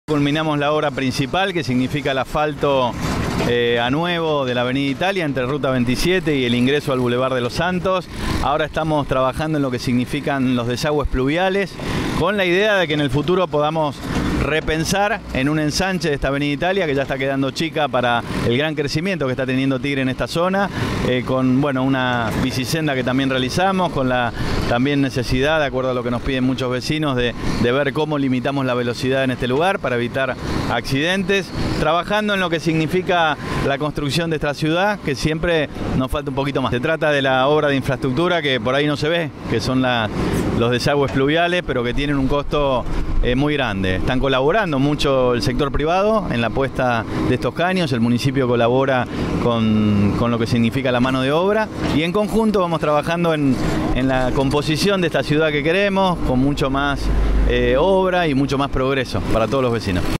Obras de entubamiento en la Avenida Italia de Nueva Delta
El intendente de Tigre inspeccionó los trabajos que realiza el Municipio en articulación con el sector privado. Las labores en los desagües contemplan 800mts entre la calle San Roque y el barrio San Andrés. Esto proporcionará una banquina más amplia y brindará mayor seguridad en la circulación de peatones y automovilistas.